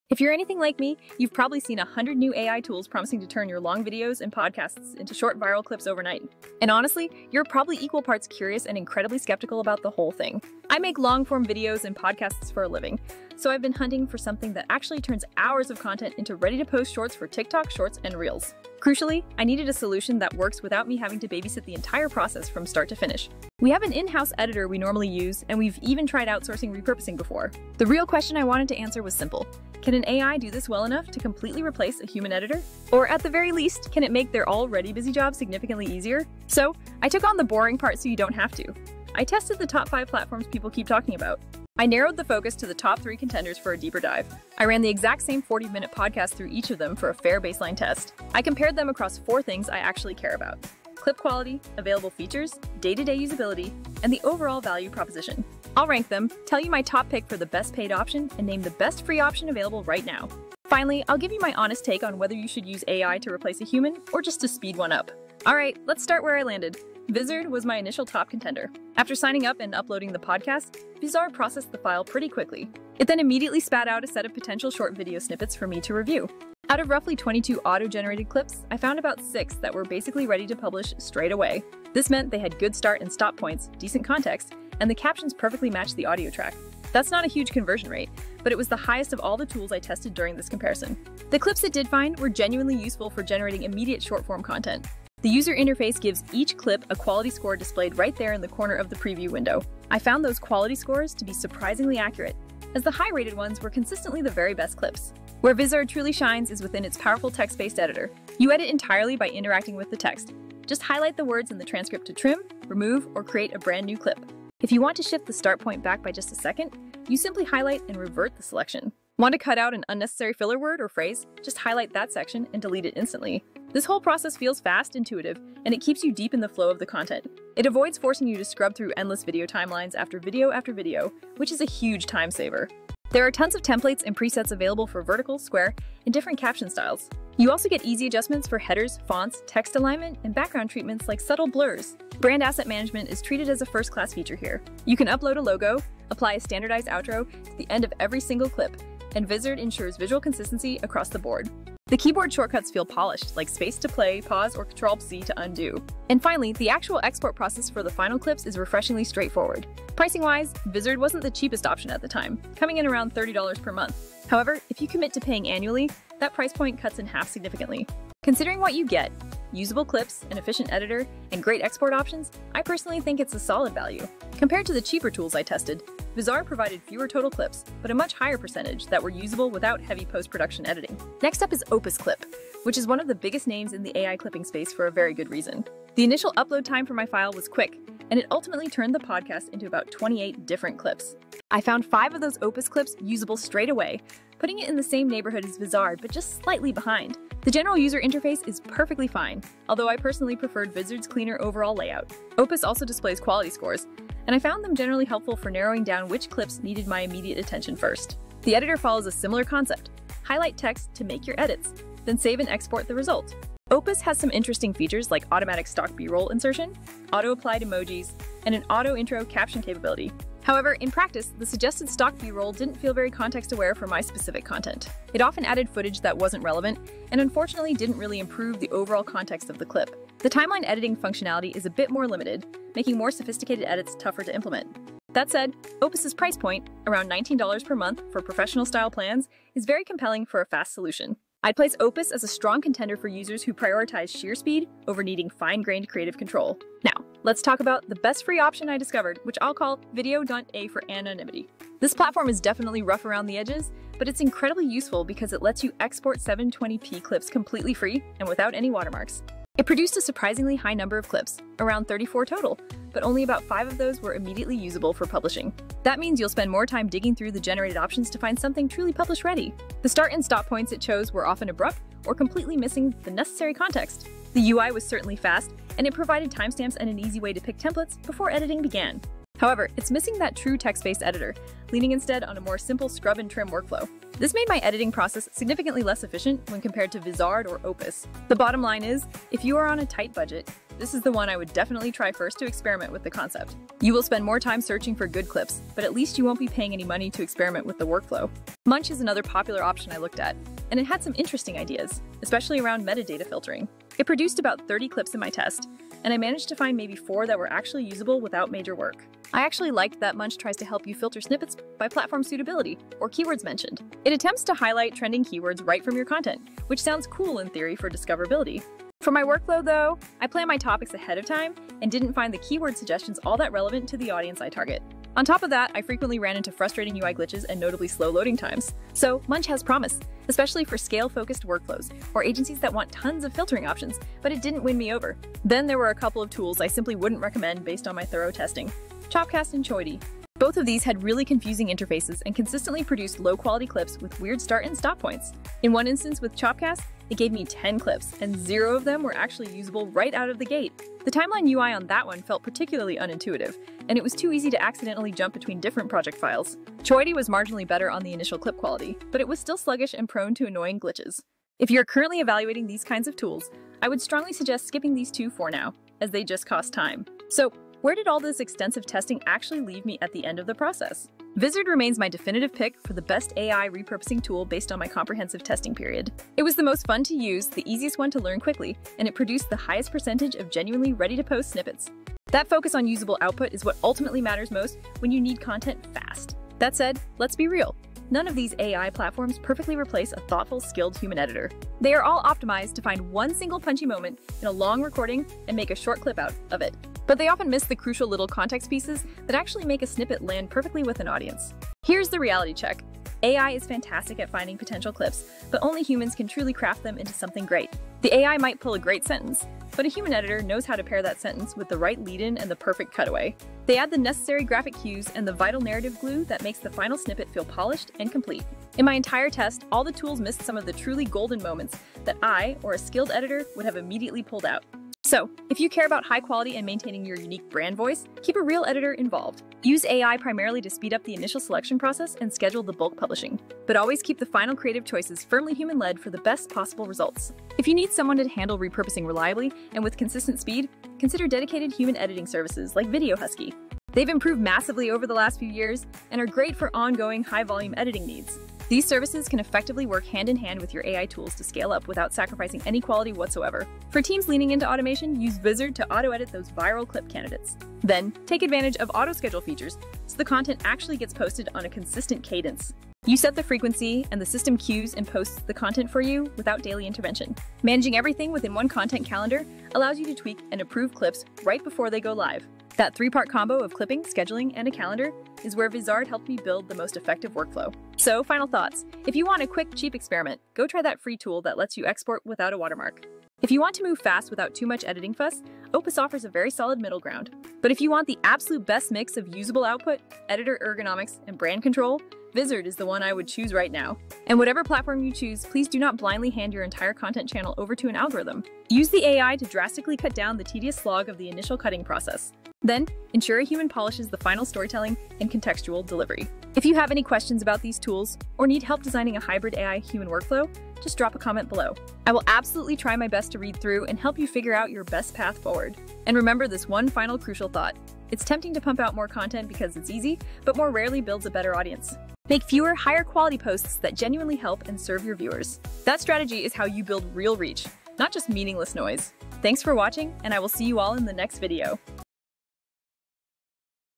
How to boost YouTube views fast: proven SEO tips & growth strategies
AI Video Editing Tools Compared: Which One's Worth Your Time?

Are AI video repurposing tools ready to replace your editor? In this video, I tested the top AI platforms that claim to turn long-form podcasts and videos into viral short clips — perfect for TikTok, YouTube Shorts, and Instagram Reels. I ran the same 45-minute podcast through 5 popular tools and compared the results across four key areas:

✅ Clip Quality
✅ Features & Editing Experience
✅ App Usability
✅ Overall Value

🚀 Here's what I found:
🏆 Best Overall:
Vizard — highest percentage of usable clips, intuitive text-based editing, rich template options, and excellent brand asset controls.
💰 Best Budget Option: Opus Clip — solid speed, decent editing, and lower cost.
🎯 Best Free Tool:A tool we’ll call “Video.A” — rough UI but gets the job done with no watermark.

I also explain why human editors still matter — especially for brand voice and storytelling — and how to blend AI with human editors for the best workflow.

🎛️ Tools reviewed:
Vizard
Opus Clip
Video.A (Free tool)
Munch
Chopcast & Choity (Not Recommended)

📌 If you're thinking about automating content repurposing, watch this first. I break down what works, what doesn't, and how to get the most out of these AI tools.